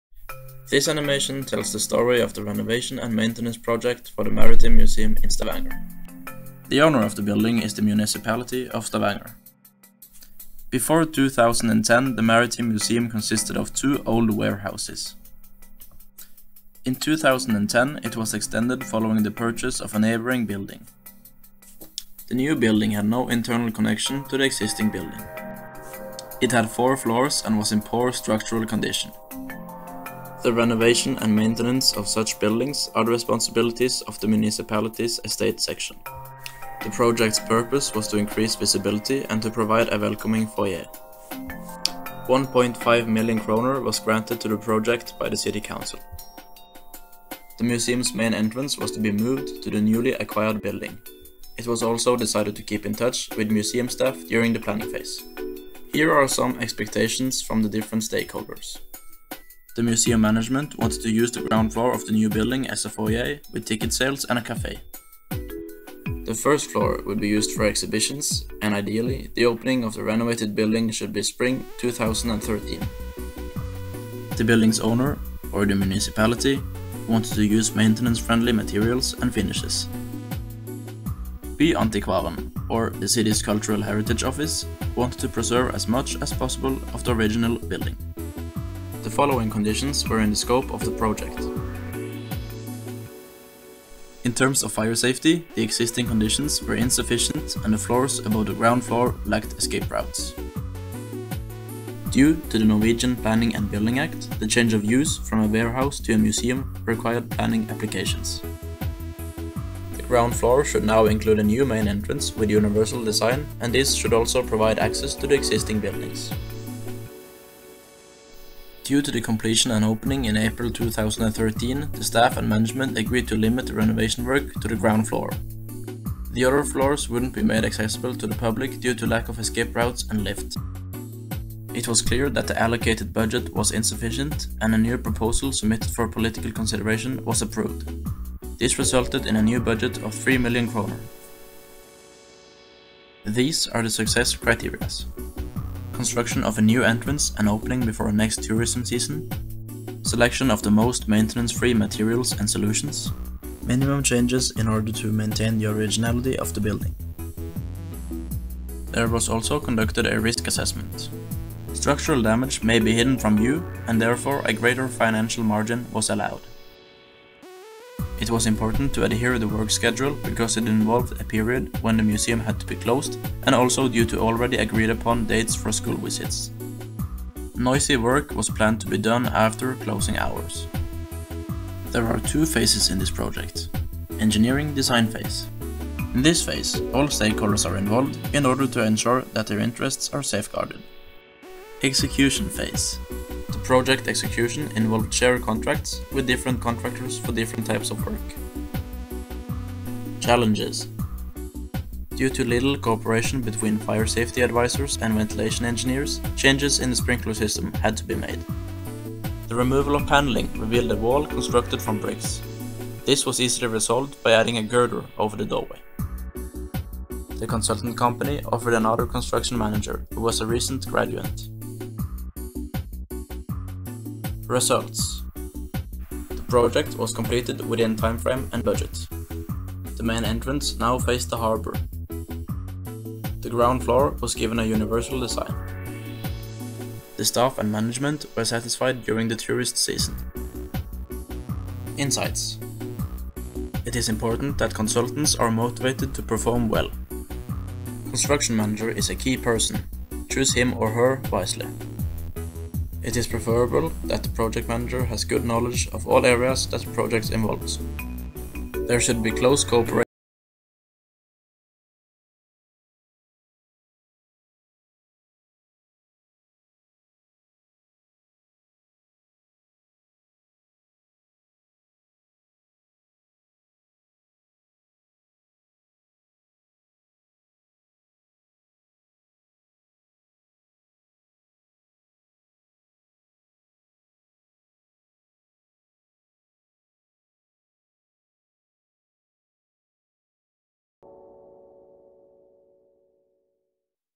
Upgrading a museum. By ProKings-2017
Animation project was an assignment that was given to the students in fall 2017. Each group had to develop a short film describing one case project from the text book (Bassam Hussein. Road to success. Narratives and insights from real life cases, 2017) The students were free to select and decide how they should make the short film. The results of this project was fascinating and I have decided to share it with you.